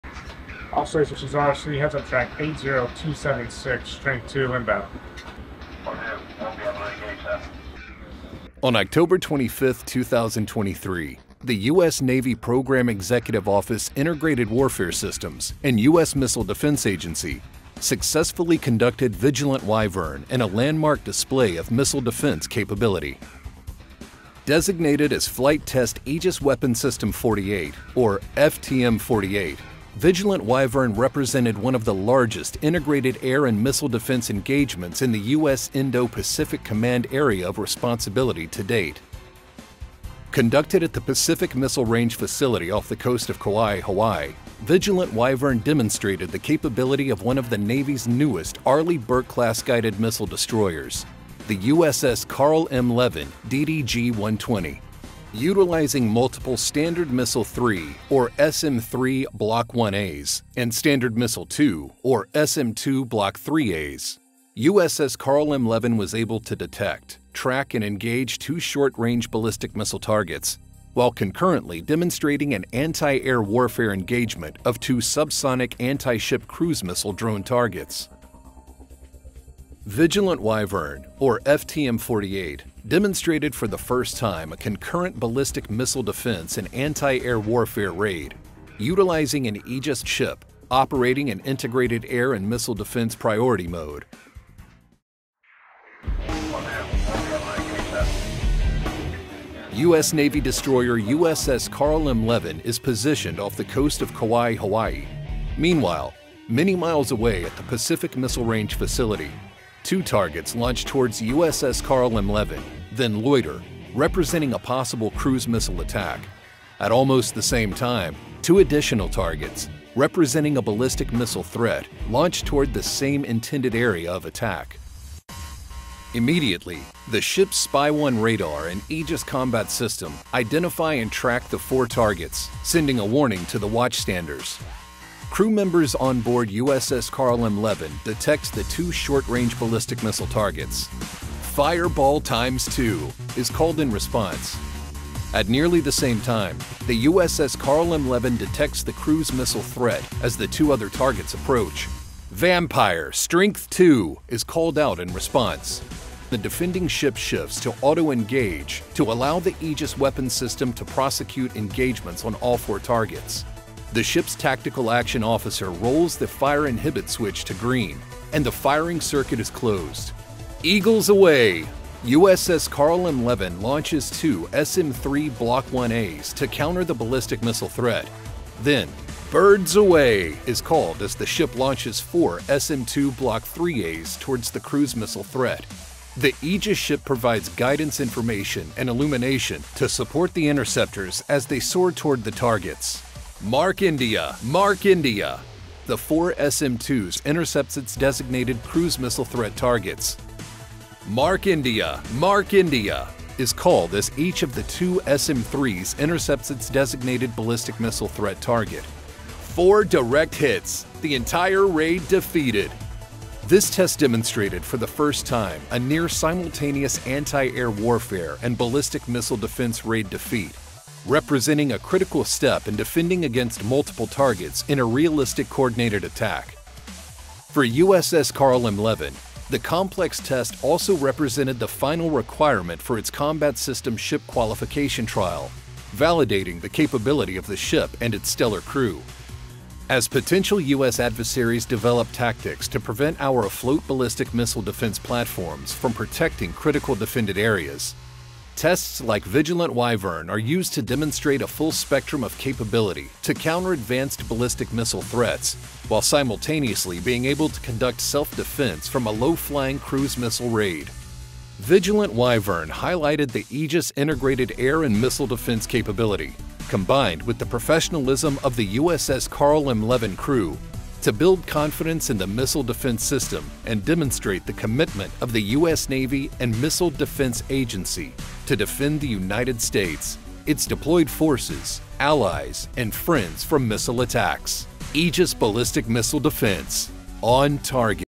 Vigilant Wyvern / Flight Test Aegis Weapon System-48 (FTM-48) Mission Video
US Navy and MDA Successfully Intercept Multiple Targets In Integrated Air and Missile Defense Test

U.S. Navy Arleigh Burke-class destroyer USS CARL M. LEVIN (DDG 120) successfully intercepted multiple targets in an Integrated Air and Missile Defense (IAMD) test executed by the U.S. Navy Program Executive Office Integrated Warfare Systems and the Missile Defense Agency (MDA) from the Pacific Missile Range Facility, Kauai, Hawaii.

The joint test, known as Vigilant Wyvern, demonstrated the capability of a ballistic missile defense-configured Aegis ship to detect, track, engage and execute intercepts of two short-range ballistic missile (SRBM) targets while concurrently demonstrating an Anti-Air Warfare (AAW) engagement of two subsonic anti-ship cruise missile drone targets.

This realistic, live-fire raid scenario represented one of the largest IAMD events ever conducted in the U.S. Indo-Pacific Command Area of Responsibility and demonstrated for the first time a concurrent Ballistic Missile Defense and Anti-Air Warfare raid.

The test, designated Flight Test Aegis Weapon System-48 (FTM-48) by the MDA, demonstrated the IAMD engagement of two SRBM targets with two Standard Missile 3 Block IA (SM-3 Blk IA) interceptors, and engagement of two subsonic anti-ship cruise missile drone targets with four SM-2 Blk IIIA interceptors.